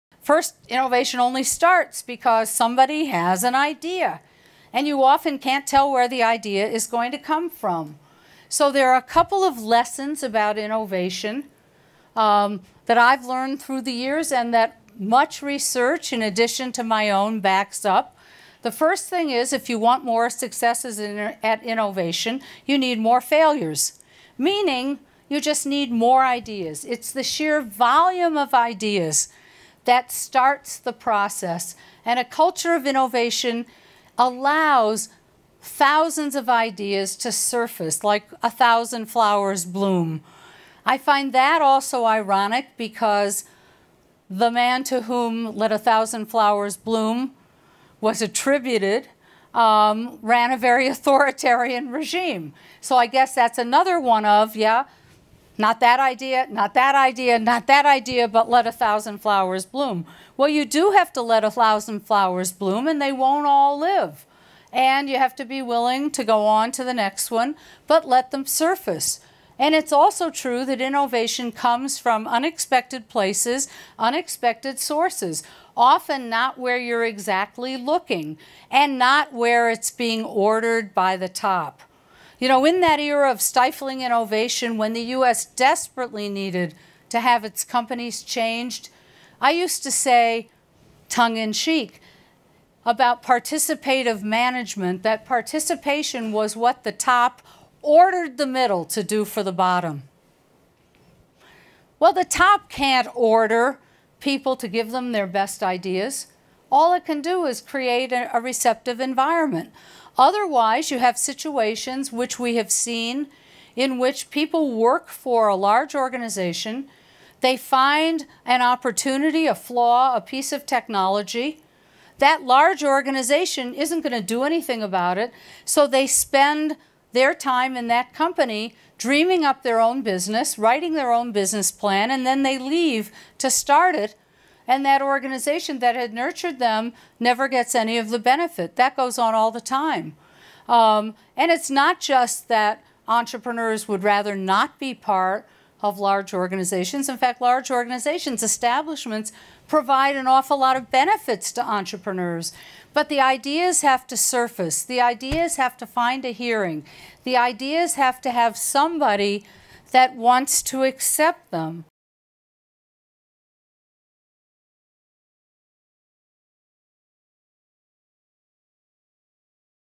Rosabeth Moss Kanter: Culture of Innovation Requires Sheer Volume of Ideas
"Change is a threat when it is done to us, but it is an opportunity when it is done by us."

Rosabeth Moss Kanter, whose strategic and practical insights guide leaders of small and large organizations worldwide, is the recipient of the 2016 Everett M. Rogers Award. In this talk, "How to Create an Innovation Culture," Dr. Kanter offers precise, practical and proven behaviors for corporations and entrepreneurs alike to ensure innovation is part of their core values and positioned for success.

Kanter has translated her wide-ranging research – on corporations and sports teams, entrepreneurs and executives, utopian communities and government agencies – into guidelines for successful and principled business practices, strategies for innovative leadership and new priorities for public policymakers. Her latest book, MOVE: Putting America’s Infrastructure Back in the Lead, lays out her ambitious agenda for a fundamental rethinking of how cities could work, going far beyond repairing our decaying transportation systems. MORE